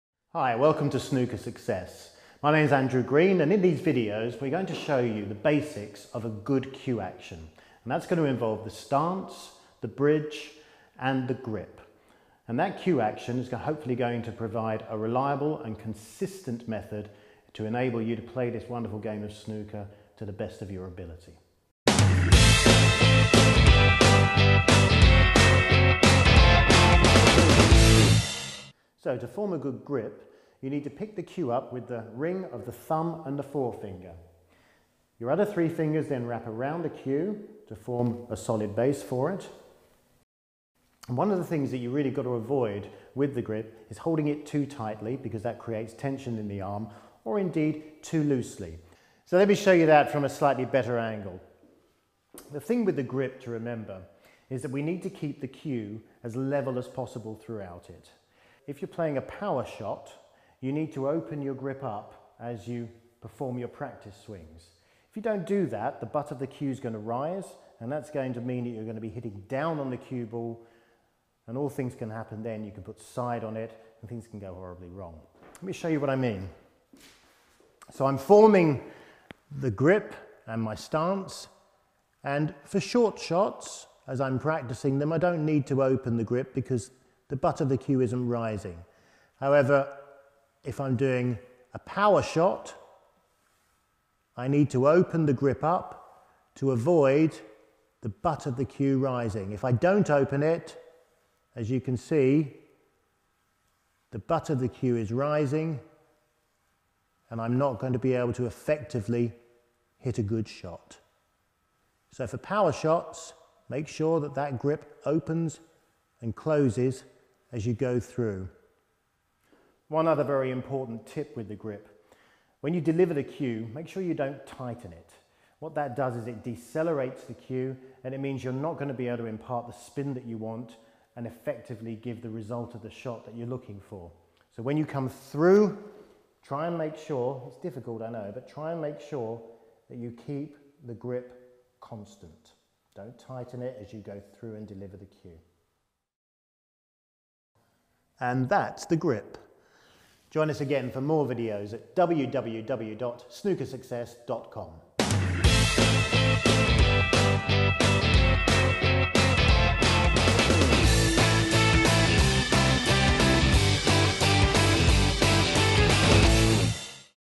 The Snooker Grip - World Snooker Coach Lessons
In this snooker coaching video we discuss the importance of a good snooker grip.

A snooker grip that is too loose or too tight can cause problems in the delivery of the cue and this short video addresses the importance of how to get a correct snooker grip.